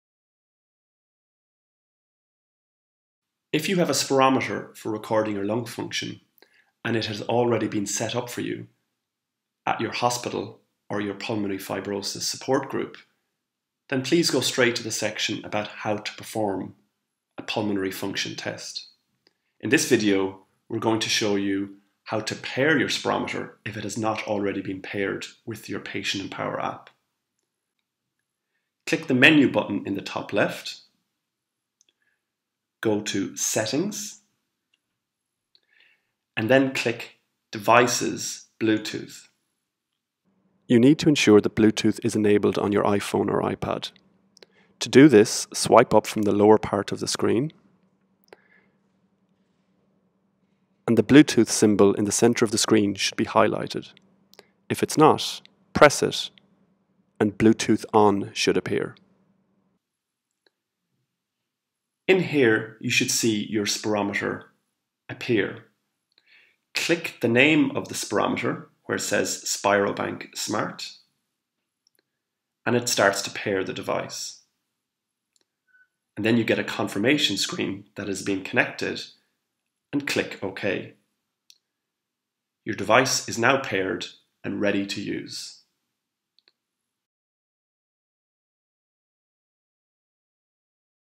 Connecting your spirometer to the patientMpower iOS app
This video shows how to connect or pair your spirometer to the patientMpower app on iPhone and iPad for performing pulmonary function tests